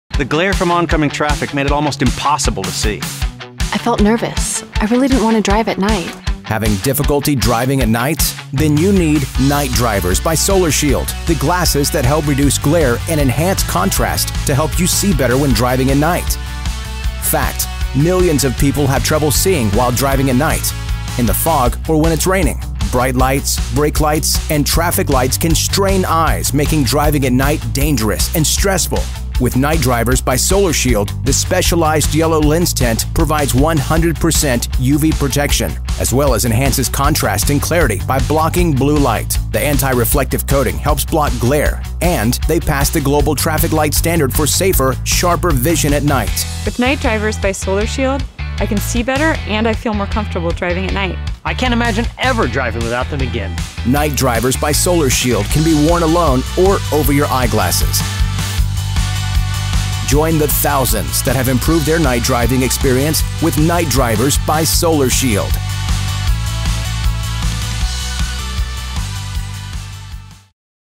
SolarShield Night Drivers
Driving at night can be dangerous. Night Drivers are the solution. Unique yellow lens enhances contrast. Anti-reflective coating blocks glare from headlights. Passes global traffic light standards. Night Drivers - More Contrast-Less Glare!